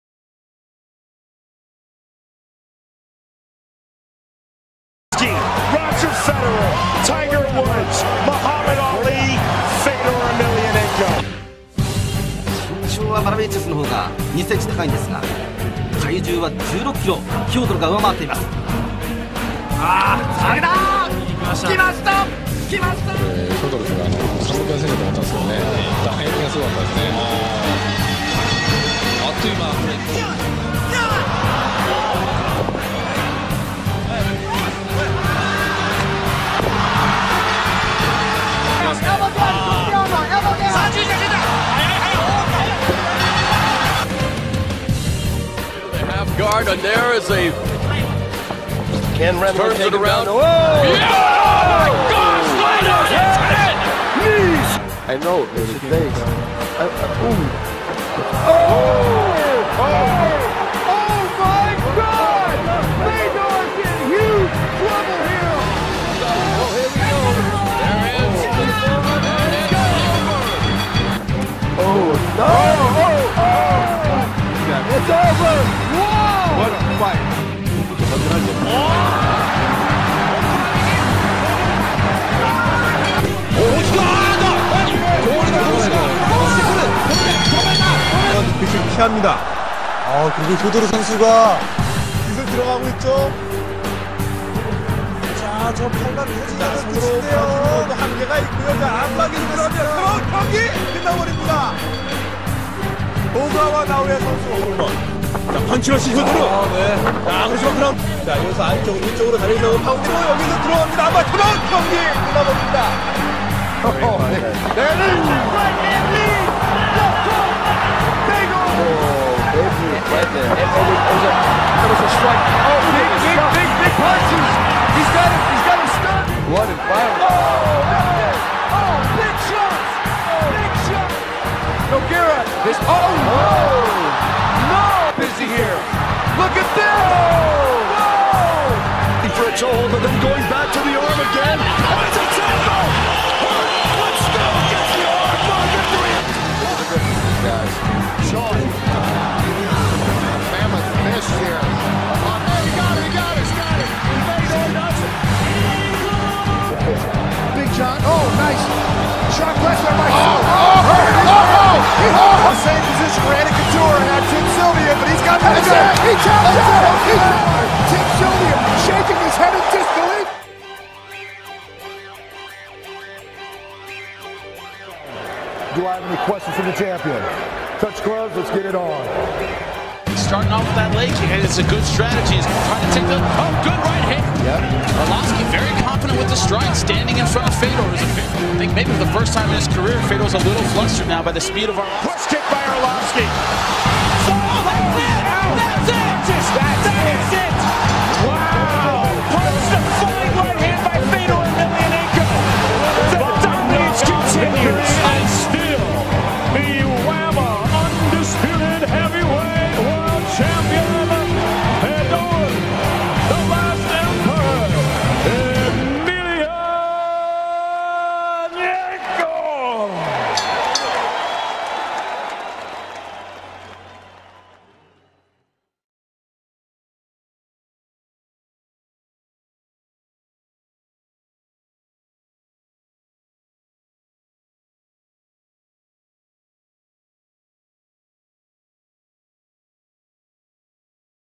IS ANDERSON SILVA THE GREATEST - WHY FEDOR EMELIANENKO IS THE GREATEST OF ALL TIME
GET OUT OF HERE HE BEAT PATRICK COTE? WOW THAILES LEITES?
THOSE ARE REALLY SCARY